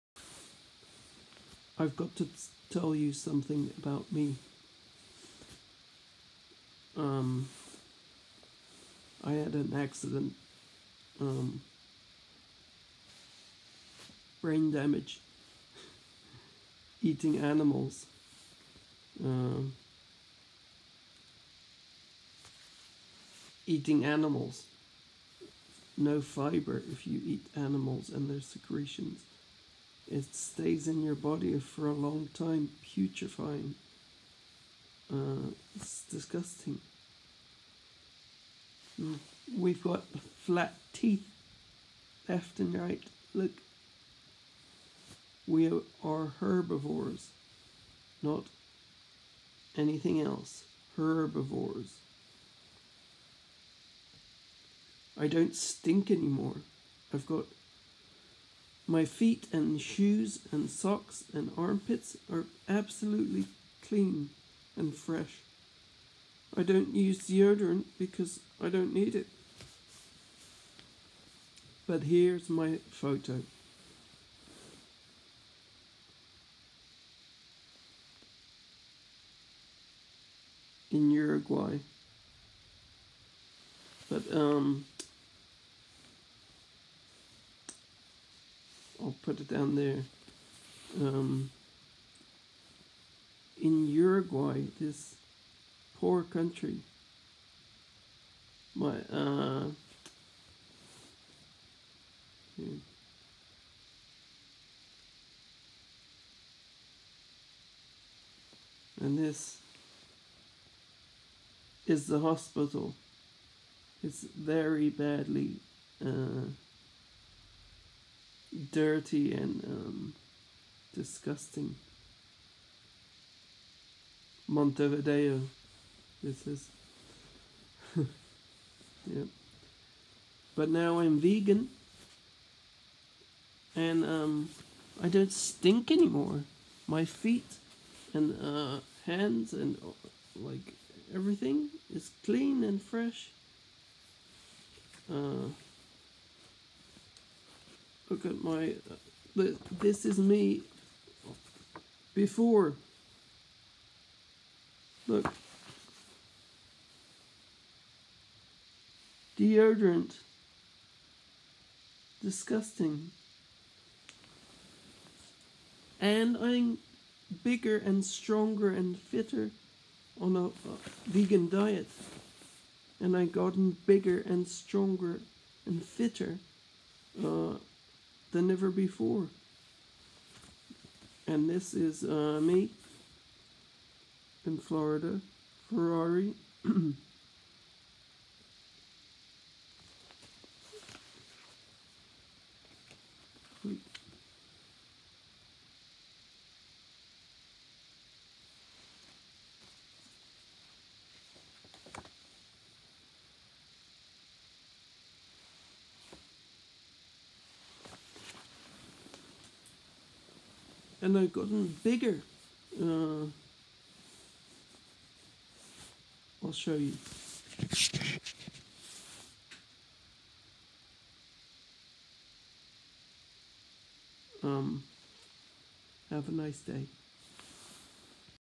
Eating rotting corpse, no fibre if you eat animals and their secretions.... 👇
Bonobos and Orangutans and Gorillas are plant-based. They never eat meat et cetera. ✅❤️😬🦷💪🐵🦍. They are 99% the same as us... 1% cancer in the wild ! We are herbivores. Scientific fact. Long long stomachs. Gorillas are huge !! 10 times as much as humans !!! Herbivores !!! Scientific fact !
 . Alzheimer’s is eating animals, fat deposits clog the arteries. We’ve got flat teeth. Little flat teeth. Moving left and right —_ 😬🦷.

. We are not Bears 🐻, or lions or tigers et cetera 😸. Where ape family. Herbivores. Scientific fact. Long long stomachs. Flat teeth. We act like ‘Omnivores’, and get heart attack which is blocked arteries eating cooorpses, and cancer and High blood pressure and diabetes and 51% death rate if you eat Cooo🧟‍♂️rpses ! No fibre if you eat animals and their secretions !!! We humans only family that’s eating animals and plaque forms. You’re not supposed to eat animals. Peer review science !!! Vegans have 4% cancer and that’s it. Lots and lots of fibre, if you eat plants and fruit and nuts and berries and tubers and lentils and beans and potatoes et cetera. Peer-reviewed science ! Six years vegan, no plaque at all. Are used to go to the dentist 6 months, plaque forms eating animals and their secretions ! PH is different 🧟‍♂️🦠💩🍔🧀🍳🍖...🔴... 🤮. I don’t need deodorant because I’m clean and fresh. My boots and socks and feet and armpits are absolutely clean and fresh. I don’t need deodorant because I’m clean and fresh !!! Try it for a month you’ll see ! And you get bigger and stronger and fitter on a plant based diet. Scientific fact ! YouTube delicious vegan food !!! So simple !